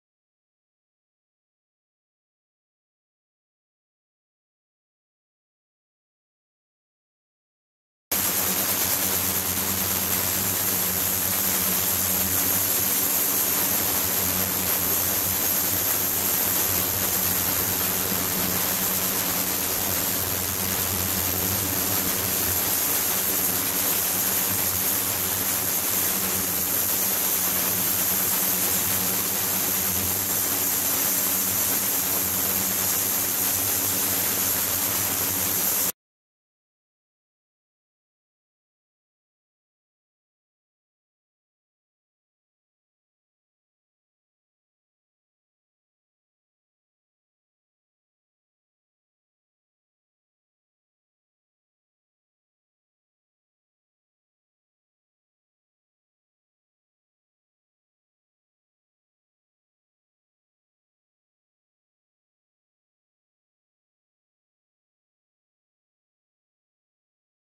超音波実験「ＵＳＷ－２８・７２Ｓ」 Ultrasonic experiment
装置：型番「ＵＳＷ－２８・７２Ｓ」

(type which controls an ultrasonic transducer (28 kHz and 72 kHz))

動画の出力状態　Output state of the video　　３３０－４００Ｗ

he nonlinearity phenomenon of an ultrasonic wave has been recognized and the effect is used.

１）対象物に有効な超音波の計測技術
２）専用の間接容器の利用技術
３）キャビテーションの制御技術
４）液循環による安定した超音波の利用技術
５）洗剤やビーズと超音波の利用技術

1) Measurement technology of an ultrasonic wave effective in a subject
2) Use technology of an indirect container for exclusive use
3) Control technology of a cavitation
4) Use technology of the stable ultrasonic wave by liquid circulation
5) Use technology of detergent, a bead, and an ultrasonic wave